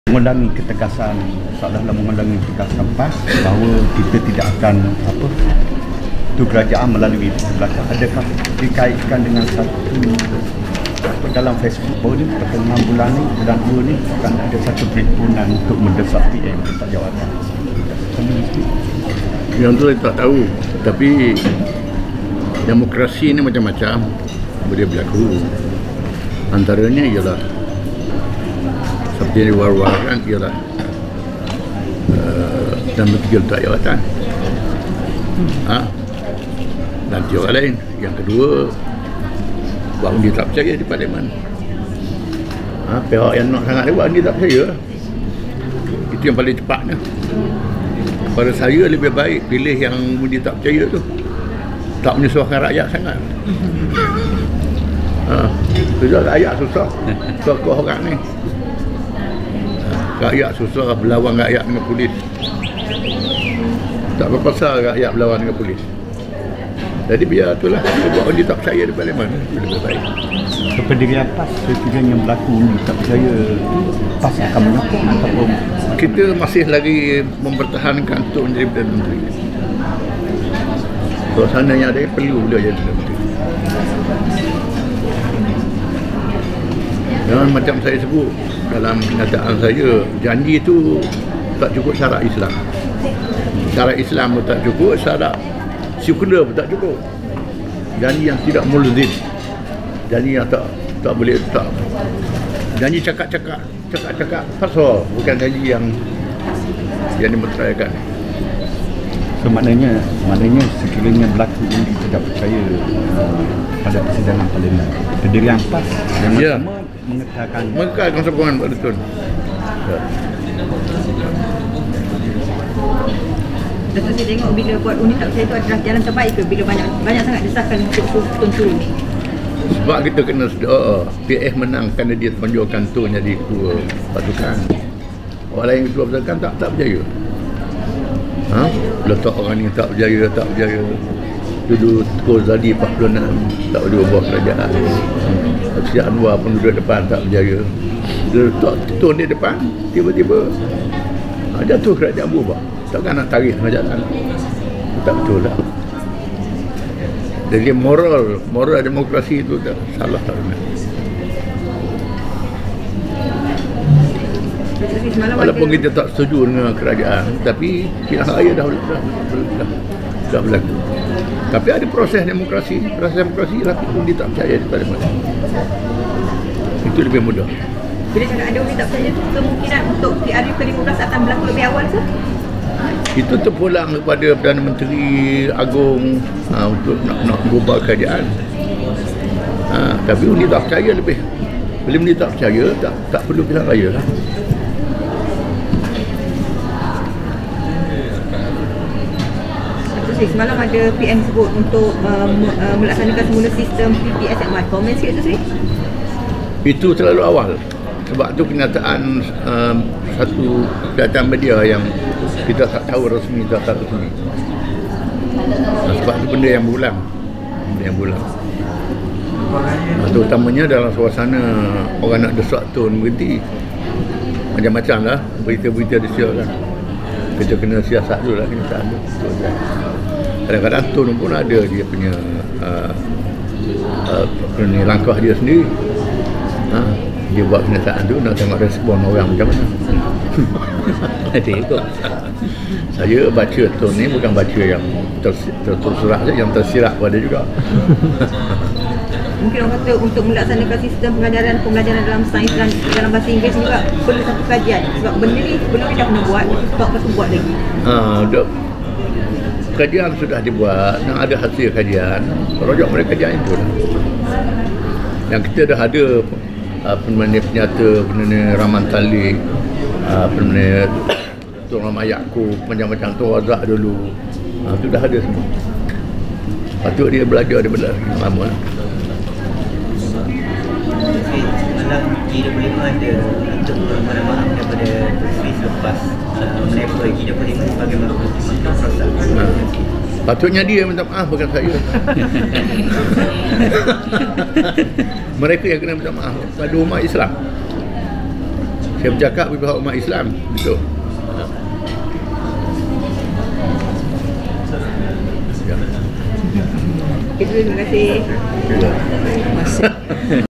Sidang Media Presiden PAS - Isu Peralihan PM, PPSMI dan G25 perlu minta maaf kepada umat Islam
Majlis Konvensyen Wanita Muda SeMalaysia

Kuala Terengganu 1 Feb 2020